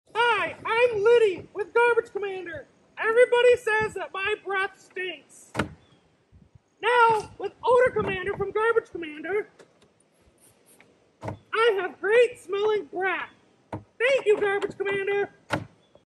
Mr. Liddy with Garbage Commander introduces the Odor Commander
Garbage Commander's Odor Commander
All Natural, Fast Acting, Powerful Odor Control Spray 22oz
Lemongrass Scent

SAFE: No toxins, no masking, no perfumes, just naturally powerful
TRUSTED: Garbage Commander's Odor Commander formula is a powerful odor eliminator that has been trusted by industry professionals for years, and is now available for home use. Get the odor out at the source!
STOPS ODORS: Odor Commander stops odors, making composting and garbage much easier to manage. Odor Commander will remove unpleasant odors from trash bins, compost, garbage cans, and much more
ELIMINATES ODORS: Eliminates household odors
ECOLOGO/UL: Certified: Certified with the EcoLogo UL program, one of Noth America's environmental Stanard and certification marks
HELP STOP ODORS: Unlike perfumes that simply mask odors, Odor Commander stops odors at the source, making composting and garbage much easier to manage

Technology provided by Sci Corp.